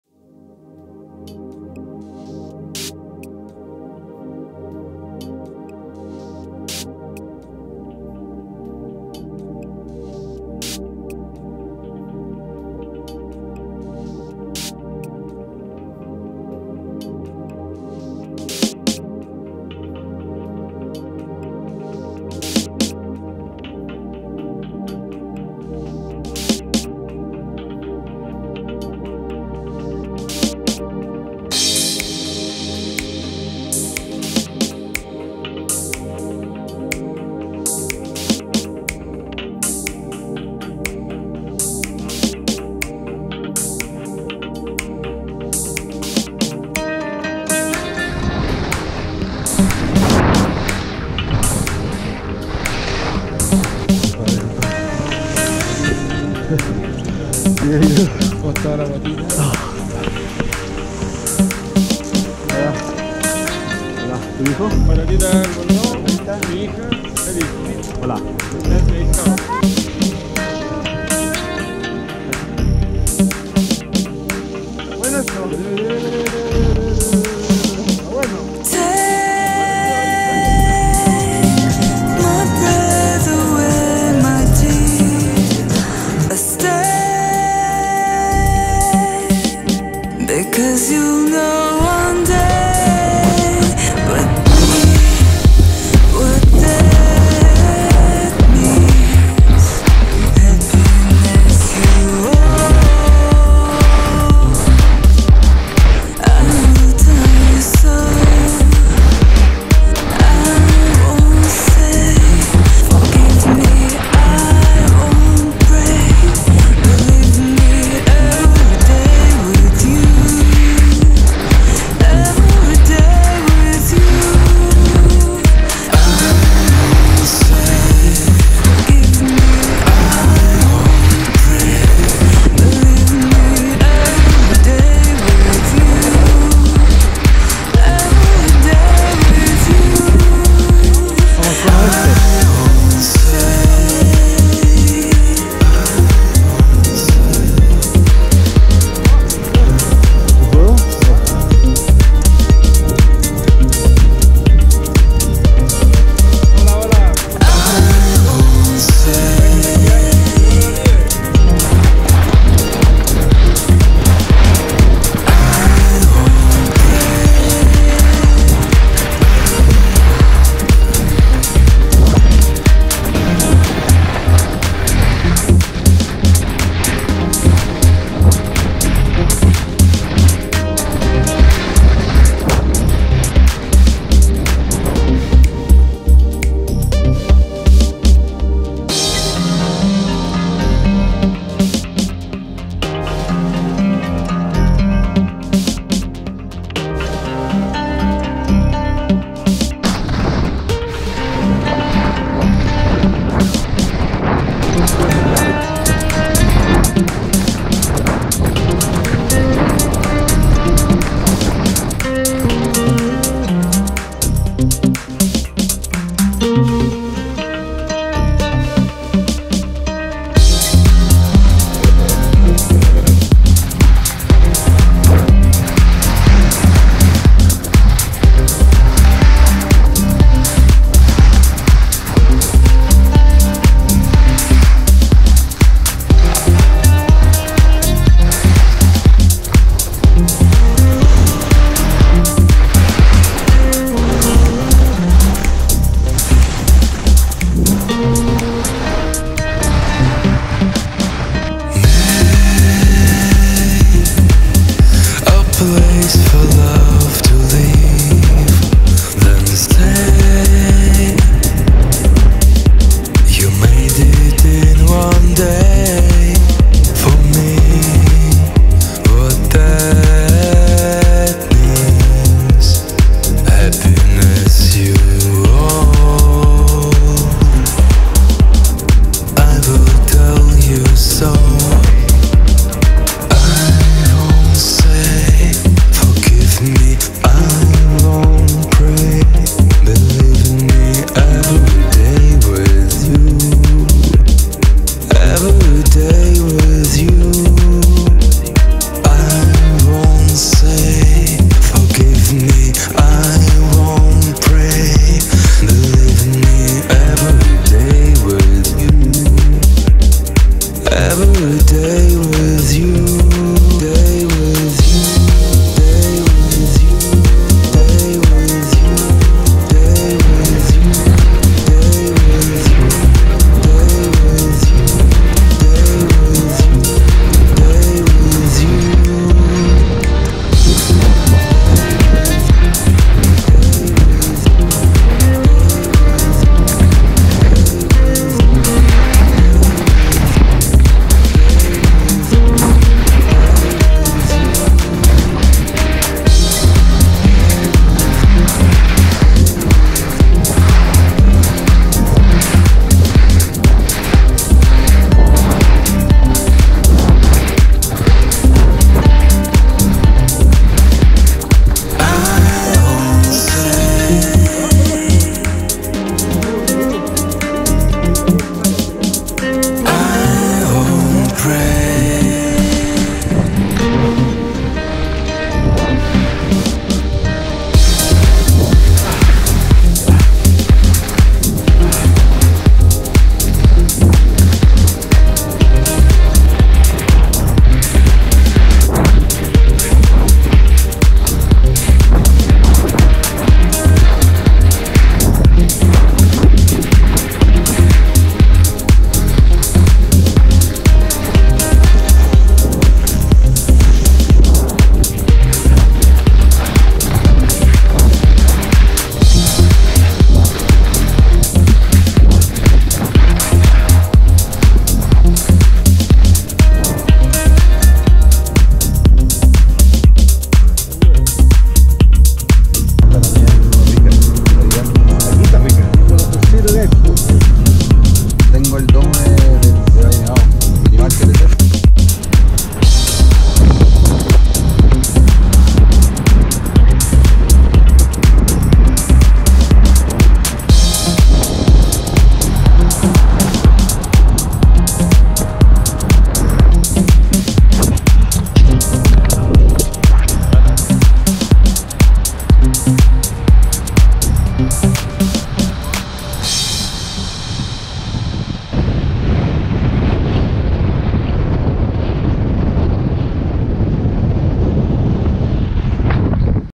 Skiing in Valle Nevado ski Colorado July 2017 Chile
My last ski trip to Valle Nevado July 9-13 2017 Chile great conditions!